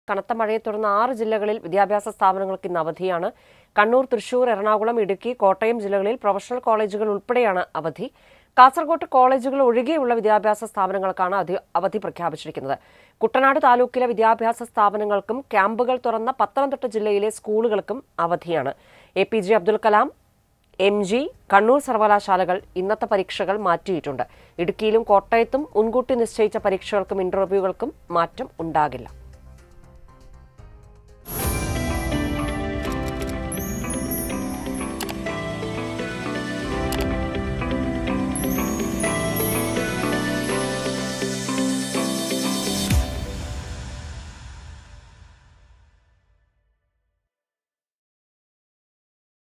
കനത്ത മഴയെ തുടർന്ന് 6 ജില്ലകളിൽ വിദ്യാഭ്യാസ സ്ഥാപനങ്ങൾക്ക് അവധി| Kerala rain alert| School | College
കനത്ത മഴയെ തുടർന്ന് 6 ജില്ലകളിൽ വിദ്യാഭ്യാസ സ്ഥാപനങ്ങൾക്ക് അവധിയാണ്; കണ്ണൂർ, തൃശ്ശൂർ, എറണാകുളം, ഇടുക്കി, കോട്ടയം ജില്ലകളിൽ പ്രൊഫഷണൽ കോളേജുകൾക്ക് ഉൾപ്പെടെയാണ് അവധി
Kerala rain alert | Heavy rain in kerala | Weather updates Kerala |School | College